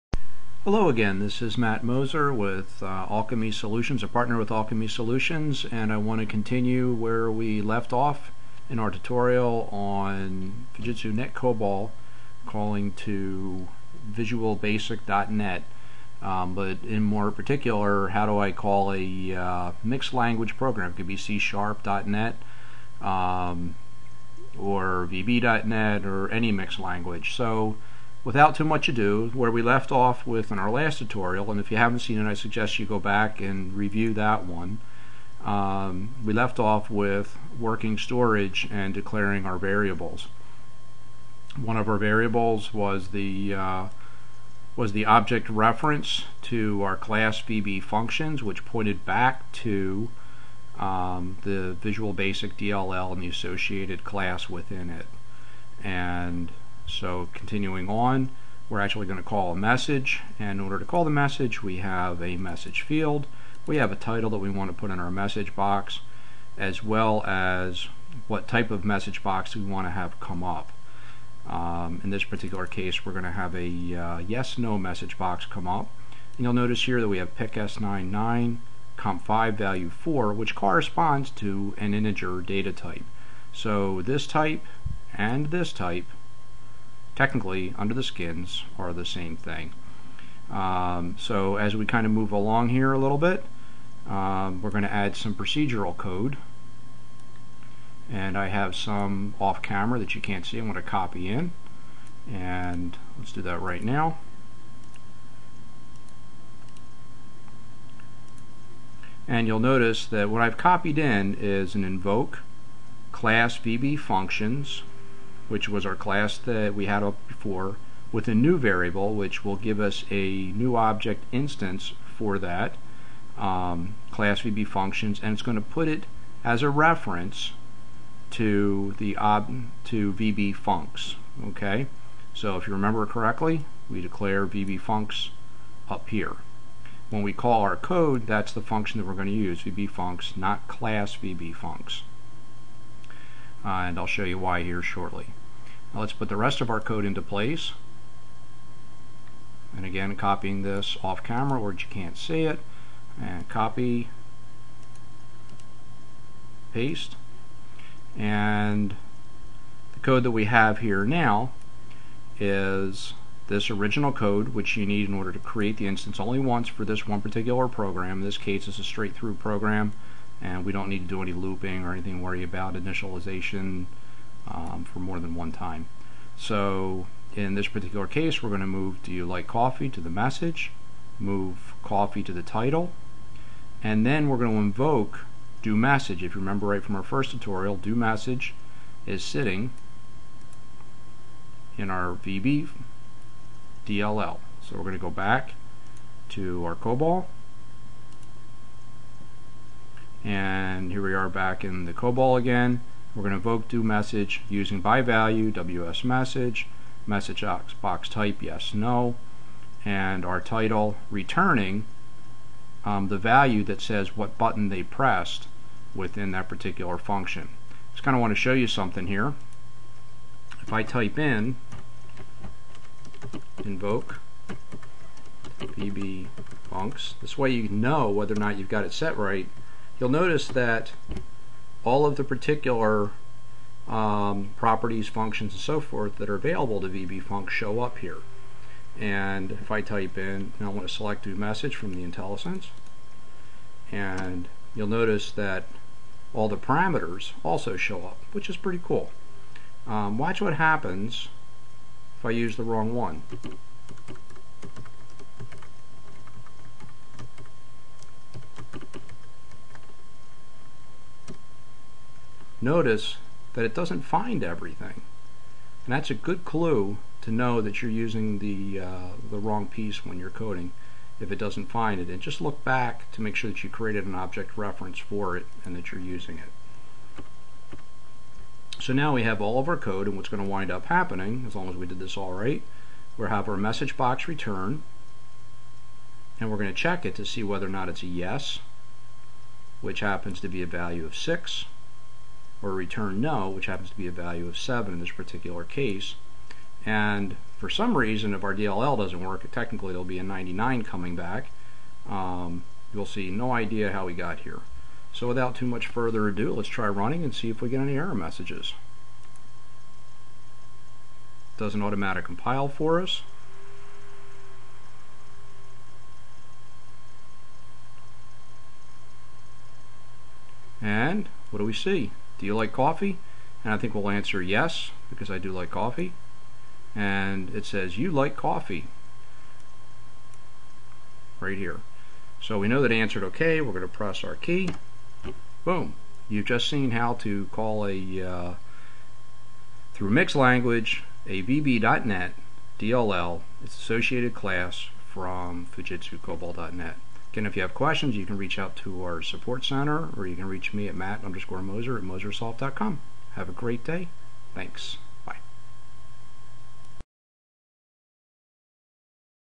NetCOBOL for .NET Tutorial: COBOL Calling VB .NET Part 2
Matt Moser NetCOBOL Tutorial covering:
- Mixed-language programming
- Example of COBOL calling a Visual Basic .NET function